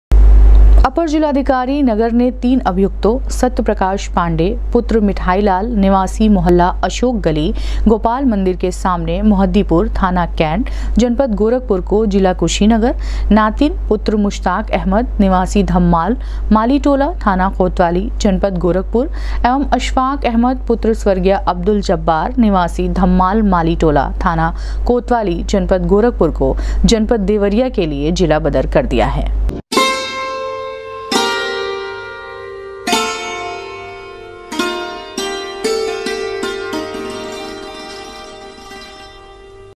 एडीएम सिटी ने 3 अभियुक्तो को किया जिला बदर, सत्य प्रकाश पाण्डेय को कुशीनगर तथा नातिन एवं अशफाक अहमद
एडीएम सिटी ने 3 अभियुक्तो को किया जिला बदर, सत्य प्रकाश पाण्डेय को कुशीनगर तथा नातिन एवं अशफाक अहमद को देवरिया के लिये किया गया जिला बदर